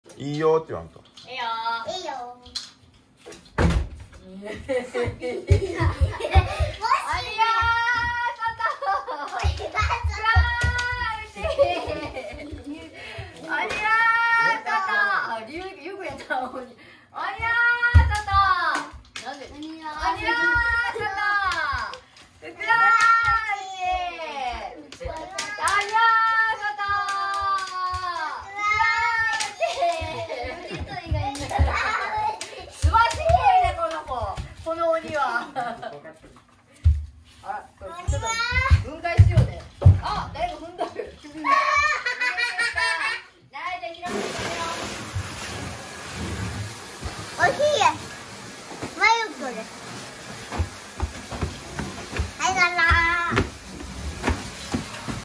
Bean-throwing〜豆まき〜
It's Setsubun, so I made a mask for the ogre out of a paper bag and threw beans.
I received a request from my fourth son that my father would be scared, so my second son will play the role of a demon this year.
I threw beans happily and enjoyed them at the end.

節分なので紙袋で鬼のお面を作って豆まきをしました
四男からお父さんがすると怖いというリクエストを受けたので、今年は次男が鬼役です
楽しそうに豆まきして最後は美味しくいただきました